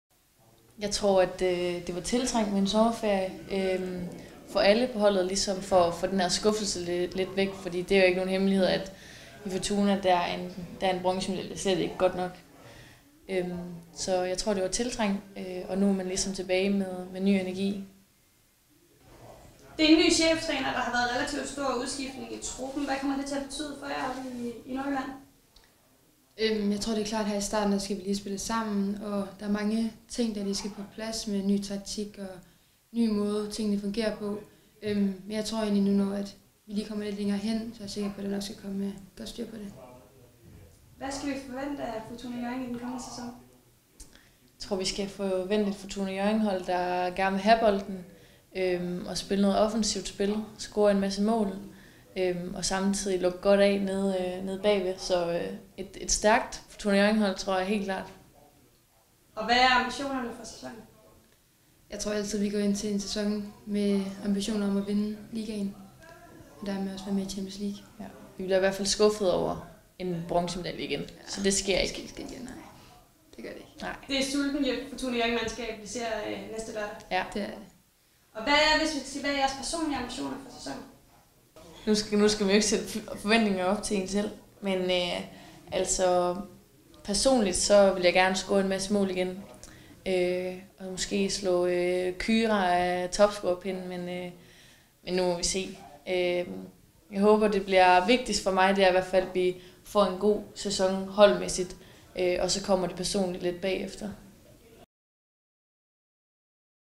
Efter en tiltrængt sommerferie, er Fortuna klar til at kæmpe om guld
Olivia Møller Holdt og Sofie Lundgaard sætter ord på en tiltrængt sommerferie og sulten efter at jagte guld til Hjørring igen.